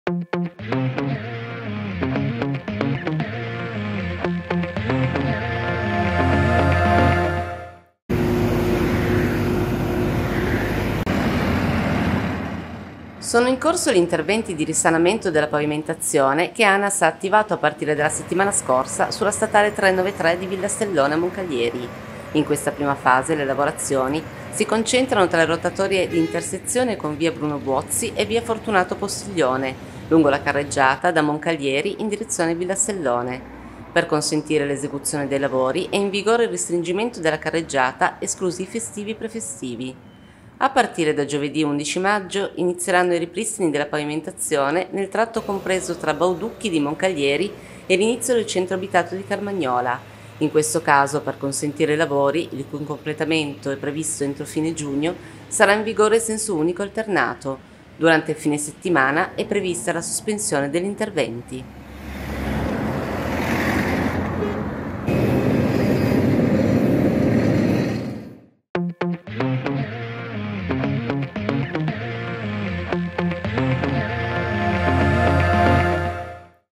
MONCALIERI - Lavori in corso sulla SS 393 di Villastellone
Sono in corso gli interventi di risanamento della pavimentazione che Anas ha attivato a partire dalla settimana scorsa sulla statale 393 “di Villastellone” a Moncalieri (TO).
In questa prima fase le lavorazioni si concentrano  tra le rotatorie di intersezione con Via Bruno Buozzi (km 1,300) e Via Fortunato Postiglione (km 3,900), lungo la carreggiata da Moncalieri in direzione Villastellone.